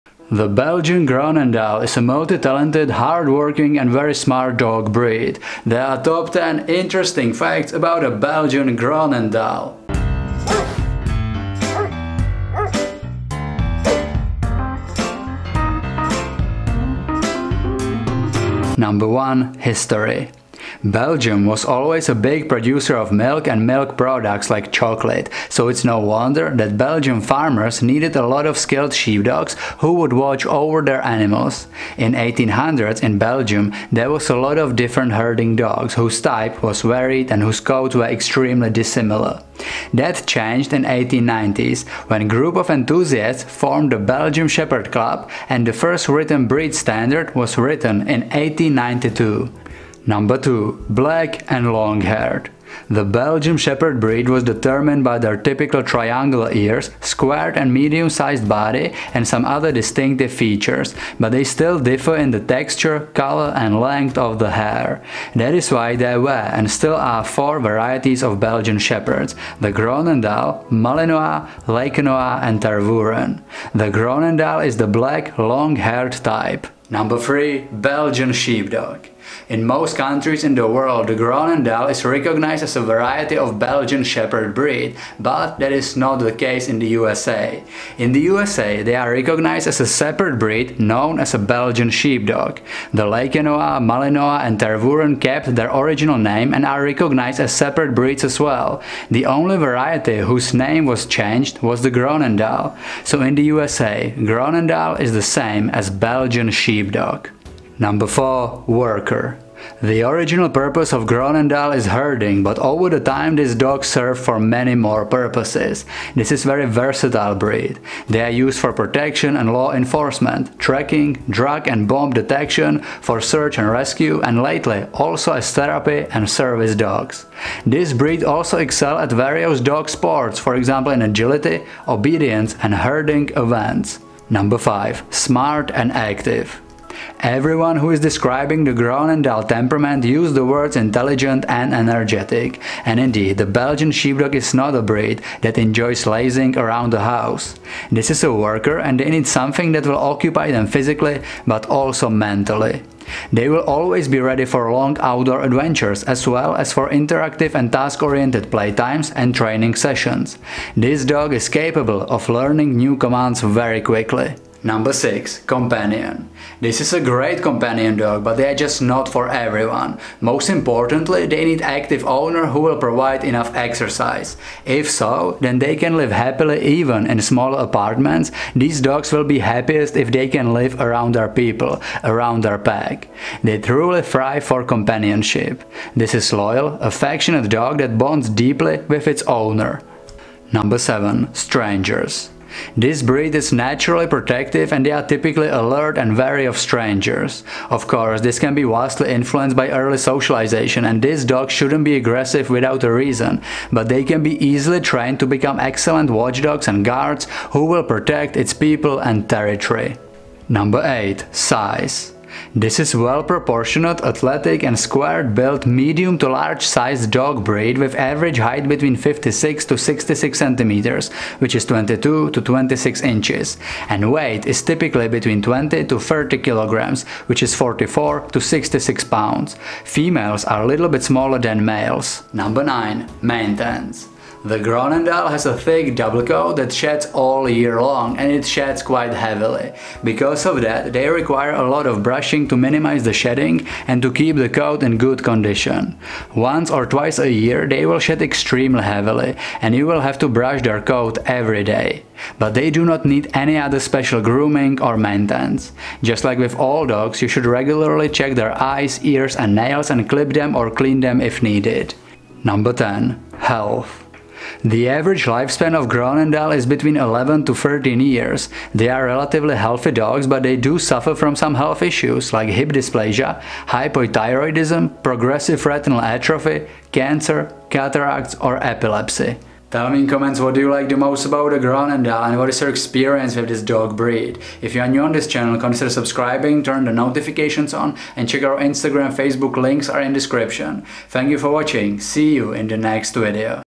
Belgian Groenendael - TOP 10 Interesting Facts - Belgian Sheepdog
Top 10 interesting facts about the Belgian Groenendael, an energetic, intelligent and affectionate dog breed.

Short Belgian Groenendael description:
The Belgian Groenendael is well proportioned, athletic and squared built medium-large sized dog breed with average height between 56-66 cm, which is 22-26 inch and weight is typically between 20-30 kg, which is 44-66 pounds. Females are little bit smaller than males.

Everyone who is describing the Groenendael temperament use the words intelligent and energetic and indeed, the Belgian Sheepdog is not a breed that enjoys lazing around the house. This is a worker and they need something that will occupy them physically but also mentally.

Belgian Groenendael facts VIDEO TIMESTAMPS:
0:19 History
0:50 Black and longhaired
1:20 Belgian Sheepdog
1:50 Worker
2:15 Smart and active
2:46 Companion
3:11 Strangers
3:33 Size
3:53 Maintenance
4:23 Health

Photo licenses:
0:01 By Seongbin Im from Seoul, Korea, Republic of - Belgian shepherd dog groenendael (벨지안 셰퍼드 독 그로넨달), CC BY-SA 2.0,
0:19 By Philco Ford - Flickr, CC BY-SA 2.0,
0:27 By localpups - groendendael-750557_1280, CC BY 2.0,
1:08 By Alexcar911 - Own work, CC BY-SA 4.0,
1:34 By Acemetrical - Own work, CC BY-SA 4.0,
1:57 By Tsaag Valren - Own work, CC BY-SA 4.0,
2:23 By Kij - Own work, CC BY-SA 4.0,
2:35 By Svenska Mässan from Sweden - Grupp 1, BELGISK VALLHUND/ GROENENDAEL, NO UCH NO V-14 SE VCH Mission Impossible Av Vikholmen, CC BY 2.0,
3:11 By Philco Ford - Flickr, CC BY-SA 2.0,
3:31 By Nyiraferrox - Own work, CC BY-SA 3.0,
4:11 By Kij - Own work, CC BY-SA 4.0,
4:21 By Томасина - Own work, CC BY-SA 4.0,
4:31 By BN - Own work, CC BY-SA 4.0,

All the other photos are mine or they are from websites like Pixabay or Unsplash and they are free for commercial use and no attribution is required